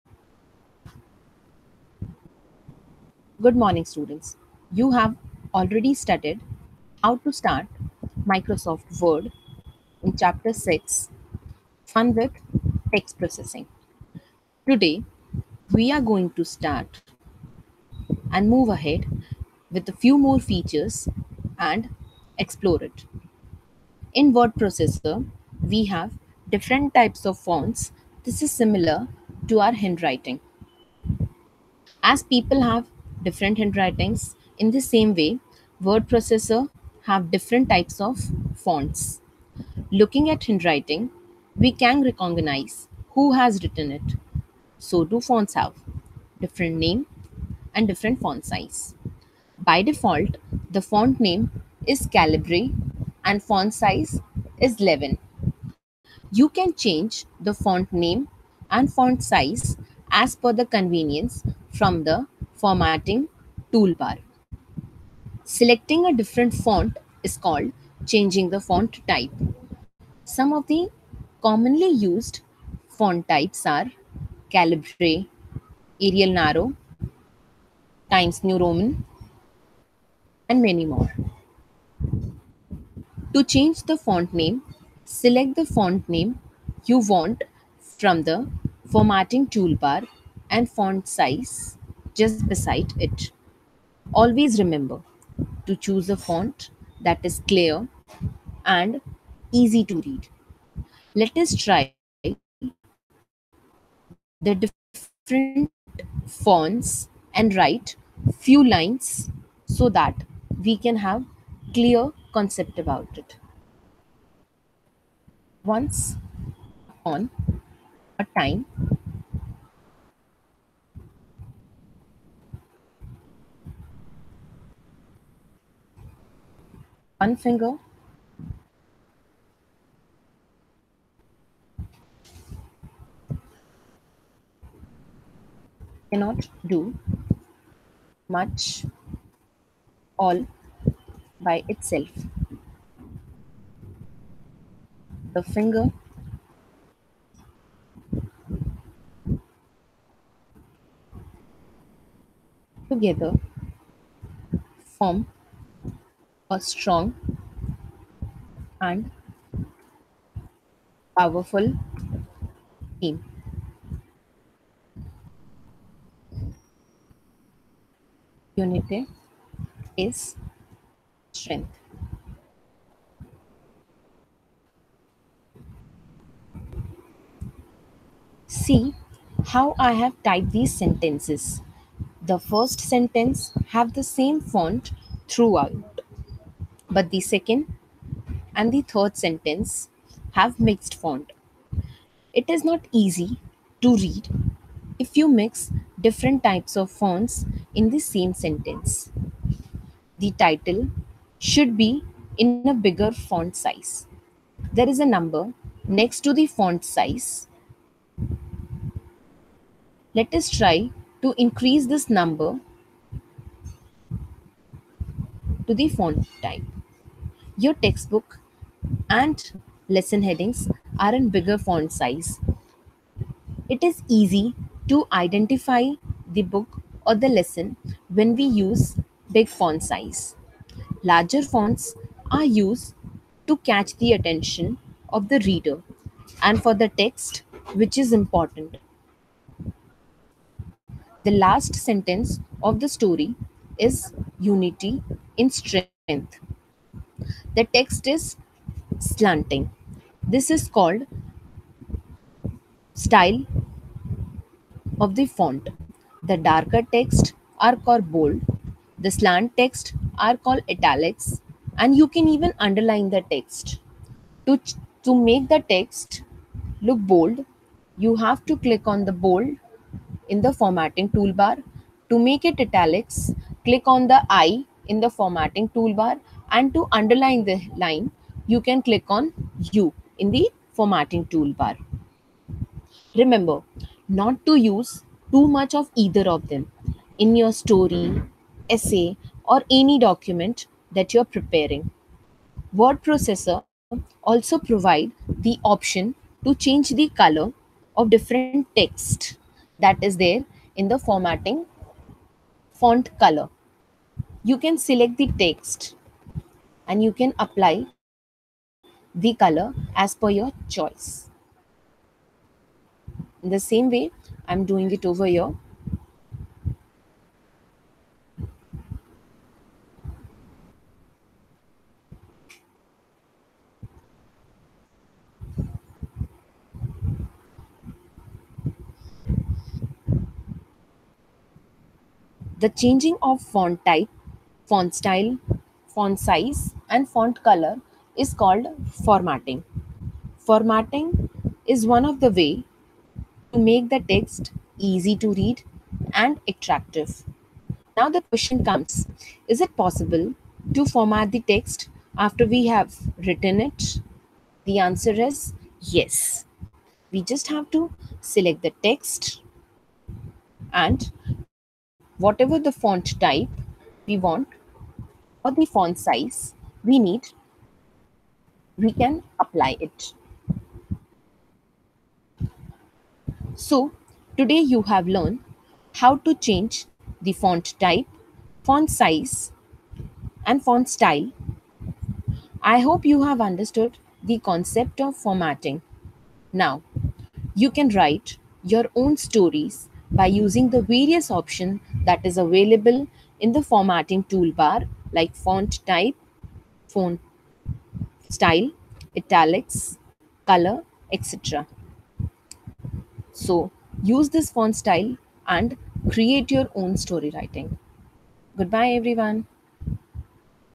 Grade 3 Chap 6 Fun with Text Processing
In this video, you will be able to learn how to format any document in Microsoft Word processor by using font type, font size, font color, and font style using the various toolbar provided in the formatting toolbar option.